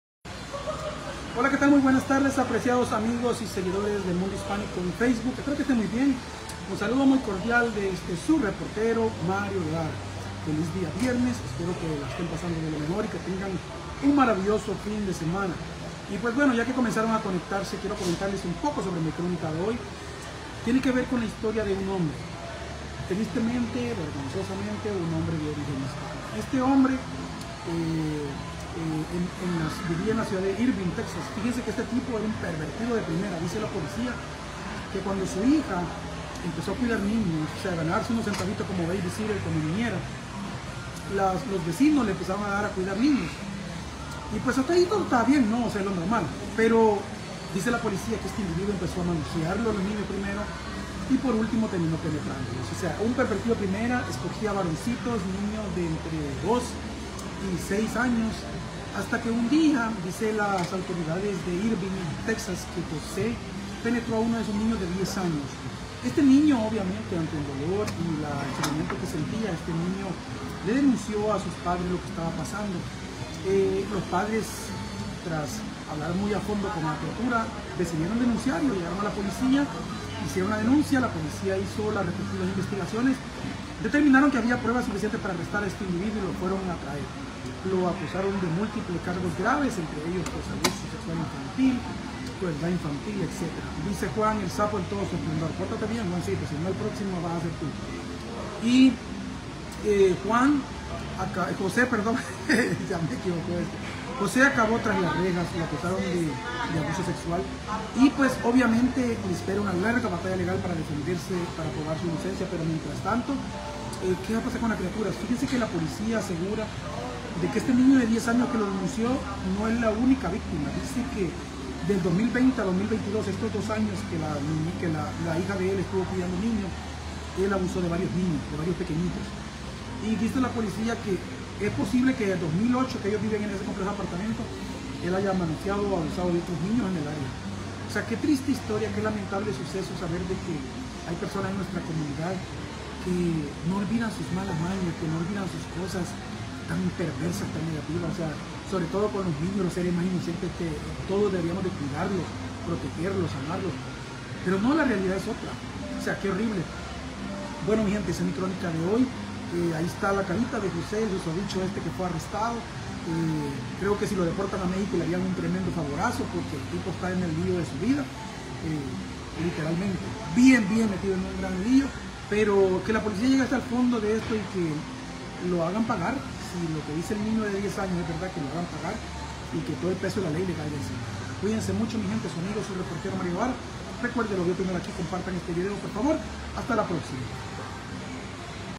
¡TRABAJABA DE BABYSITTER Y ÉL TRAICIONÓ LA CONFIANZA! Hispano se aprovechó del trabajo de su hija
¡SU HIJA TRABAJABA DE BABYSITTER Y ÉL TRAICIONÓ LA CONFIANZA! José Aguado Robles se aprovechó del trabajo de su hija para satisfacer sus oscuros deseos ¿Qué se merece este hispano?